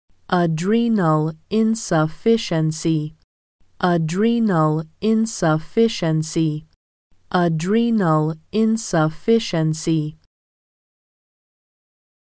Pronounce Medical Words ― Adrenal Insufficiency
This video shows you how to say Adrenal Insufficiency.

How would you pronounce Adrenal Insufficiency?